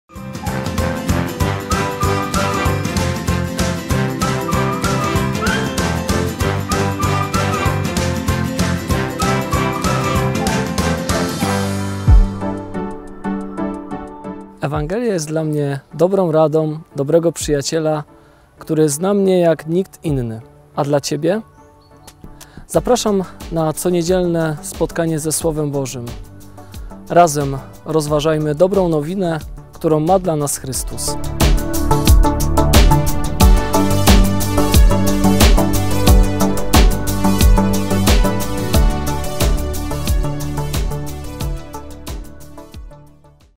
Dobra Nowina - zaproszenie
Jak często sięgasz po Pismo Święte? Czy rozważasz Ewangelię, którą słyszysz podczas niedzielnej Mszy świętej, a może ciągle brakuje Ci na to czasu? Seria podcastów „Dobra Nowina” to szansa dla każdego z nas. Razem wsłuchujmy się w Słowo, które kieruje do nas Pan.

realizacja i montaż: Karolina Zając, Piotr Ćwik